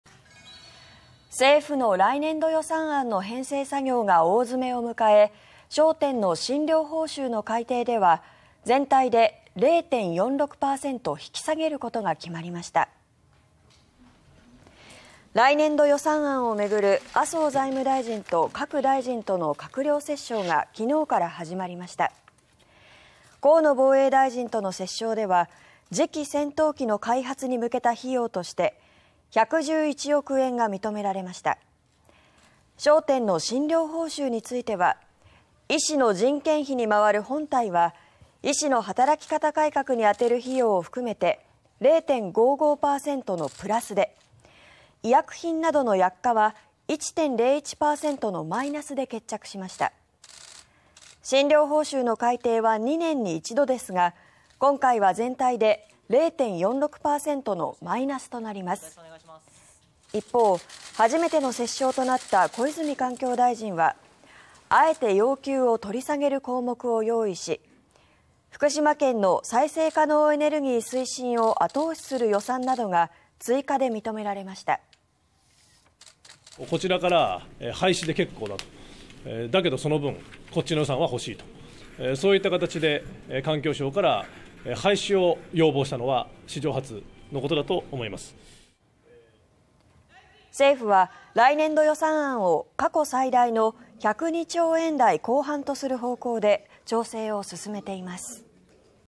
来年度予算案 診療報酬全体で0.46％引き下げ(19/12/18)
政府の来年度予算案の編成作業が大詰めを迎え、焦点の診療報酬の改定では全体で0.46％引き下げることが決まりました。

　来年度予算案を巡る麻生財務大臣と各大臣との閣僚折衝が、17日から始まりました。河野防衛大臣との折衝では次期戦闘機の開発に向けた費用として111億円が認められました。焦点の「診療報酬」については、医師の人件費に回る「本体」は医師の働き方改革に充てる費用を含めて0.55％のプラスで、医薬品などの「薬価」は1.01％のマイナスで決着しました。診療報酬の改定は2年に1度ですが、今回は全体で0.46％のマイナスとなります。一方、初めての折衝となった小泉環境大臣は、あえて要求を取り下げる項目を用意し、福島県の再生可能エネルギー推進を後押しする予算などが追加で認められました。
　小泉環境大臣：「こちらから『廃止』で結構だと。だけど、その分こっちの予算は欲しいと。そういった形で環境省から『廃止』を要望したのは史上初のことだと思います」
　政府は来年度予算案を過去最大の102兆円台後半とする方向で調整を進めています。